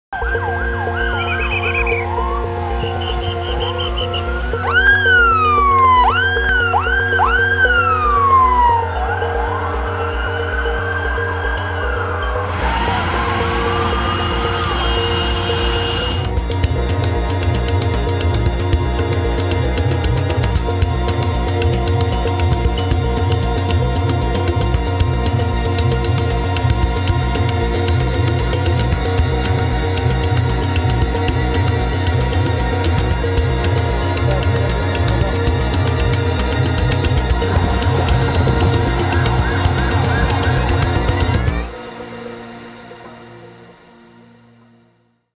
Car bomb kills 2 in southern Turkey
A car bomb targeting a Turkish government building in the southern city of Adana on Nov. 24 killed at least two people and wounded 16 others.